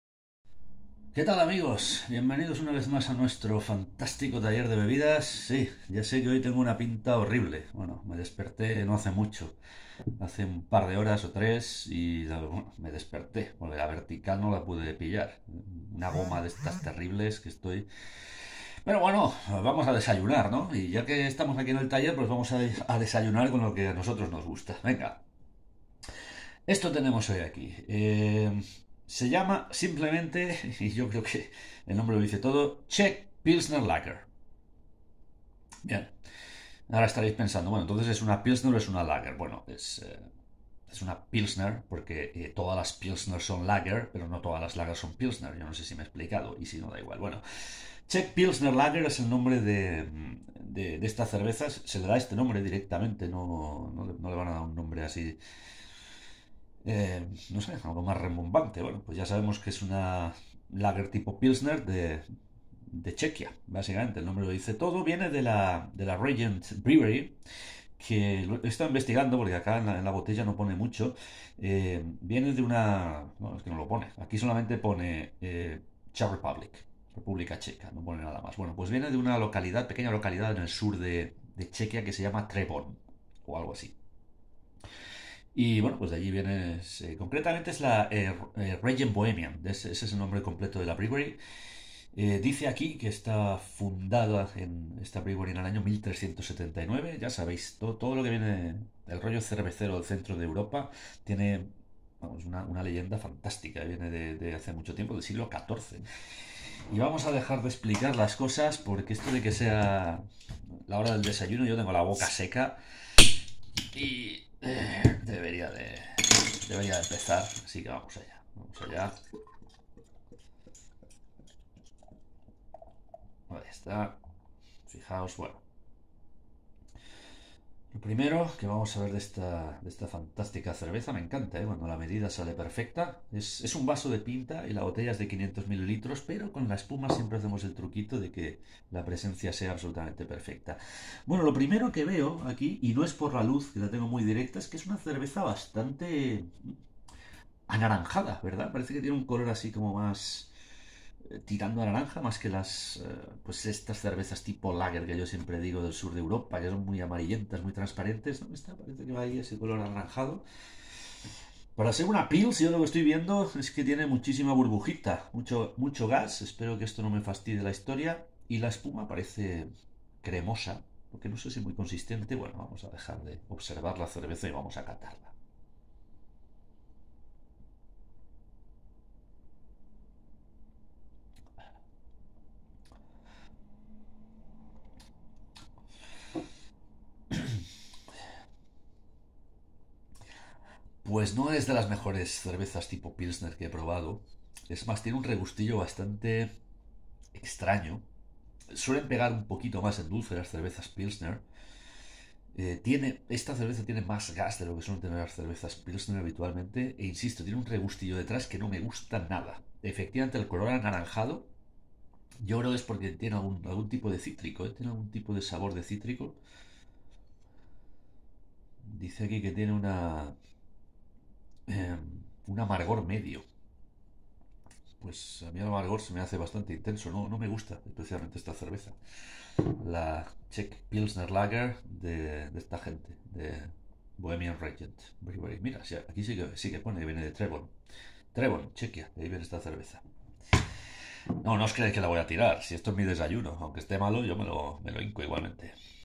Czech Pilsner Lager 🇨🇿
Cerveza de Třeboň, Chequia. Tipo pilsner. 5.0% de alcohol.